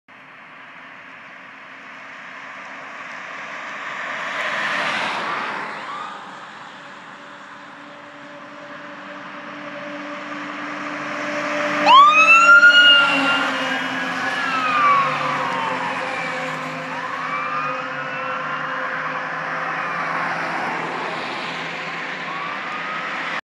Amb. 20A responding
Went in service after arriving an responded to another MVA.